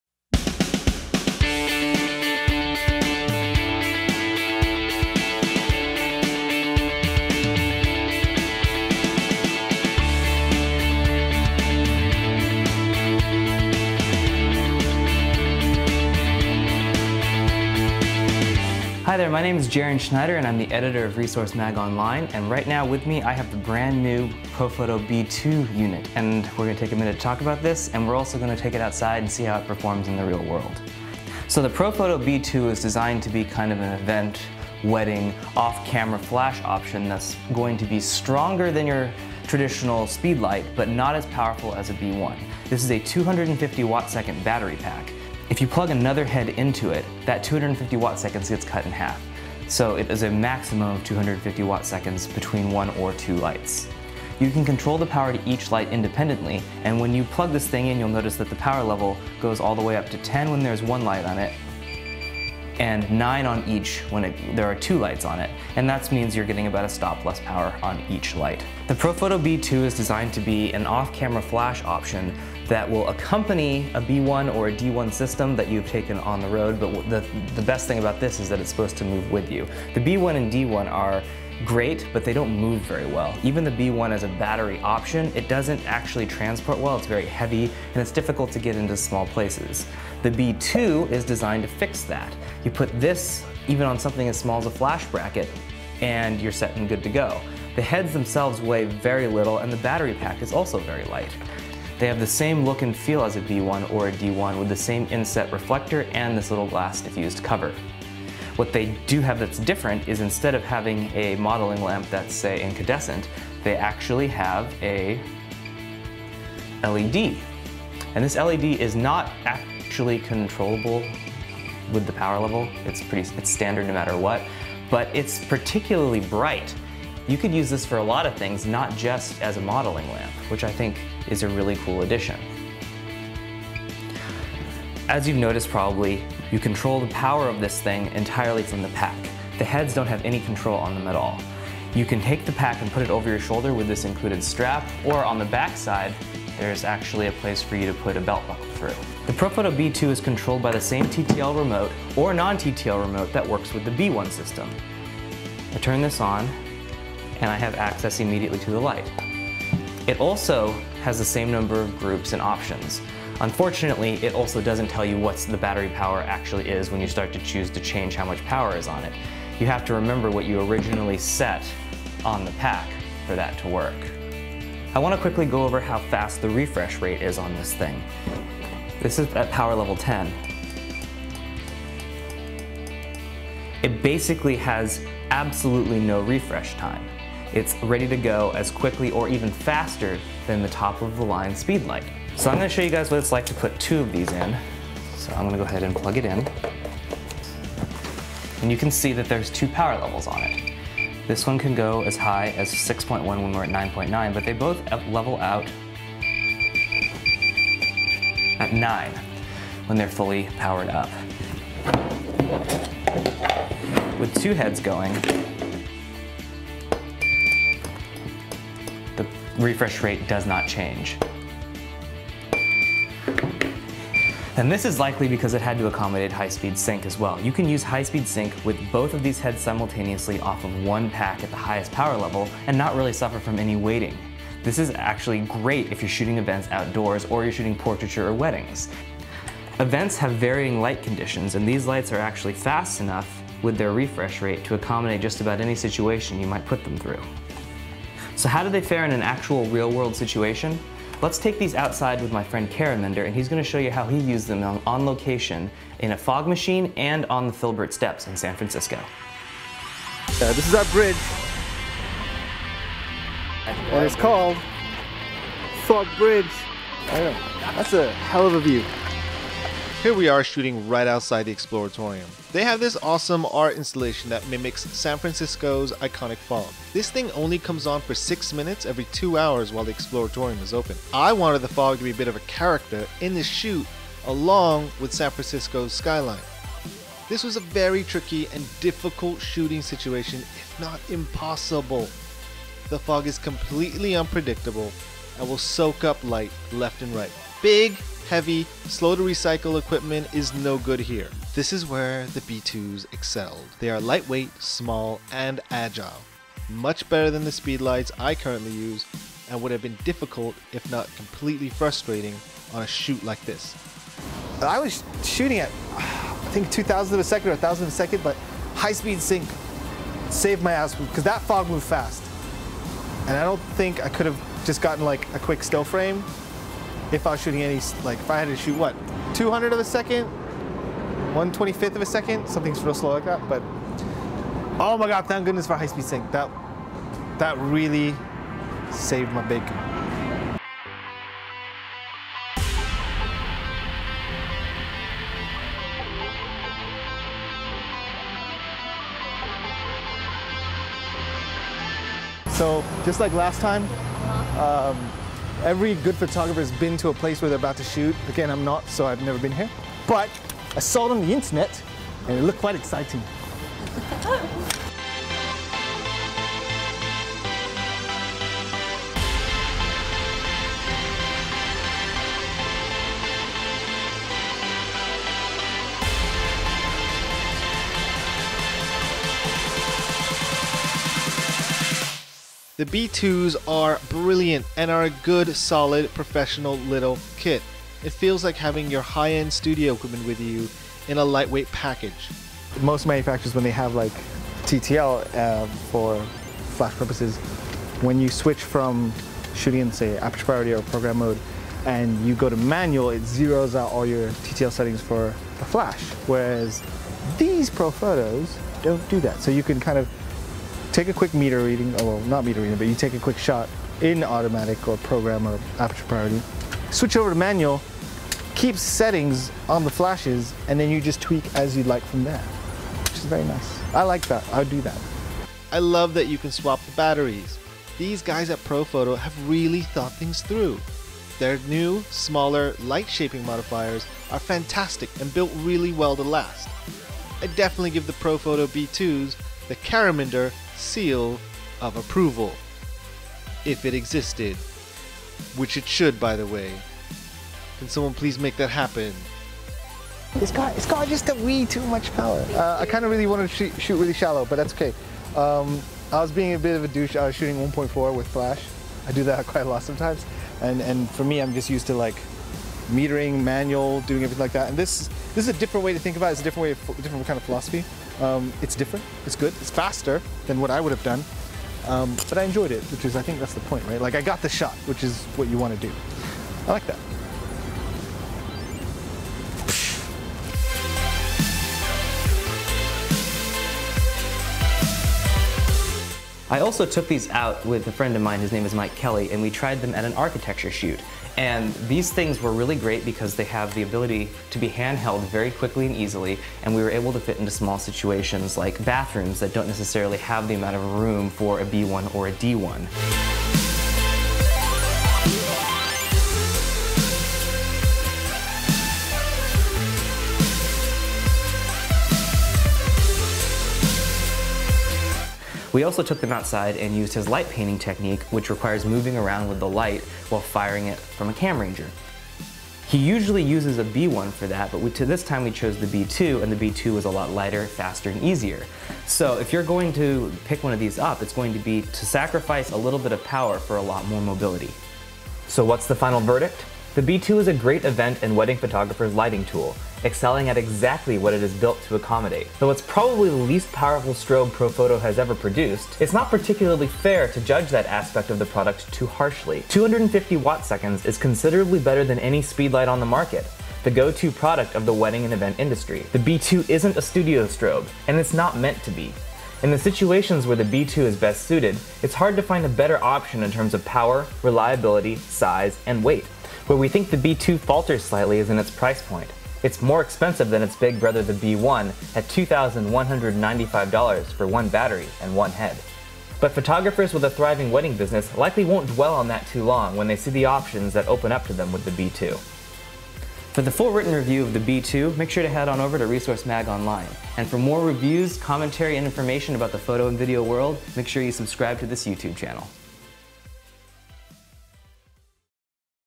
Profoto B2 OCF Battery Strobe Review
Jaron Schneider and Karaminder Ghuman review the new Profoto B2 OCF (Off Camera Flash) battery strobe. Guest appearance by architectural photographer Mike Kelley. Read the full review